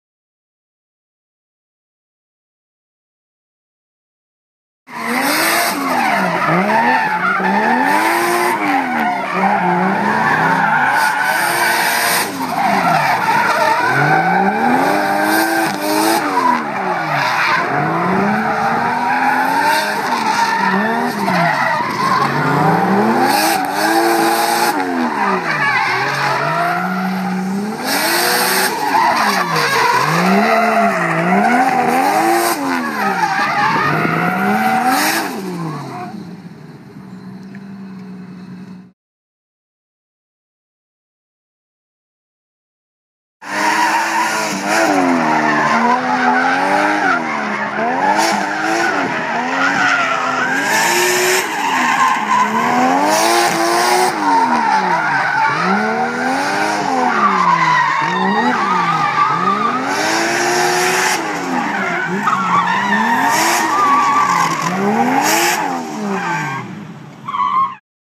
Motorkhana Rd 2 2013 - Accordian and Diamond Slalom (Mirrored)
By this time in the day the welds in the diff had broken and it wasn't locking up consistently. It mostly affected the entry into each turn but didn't bother me too much.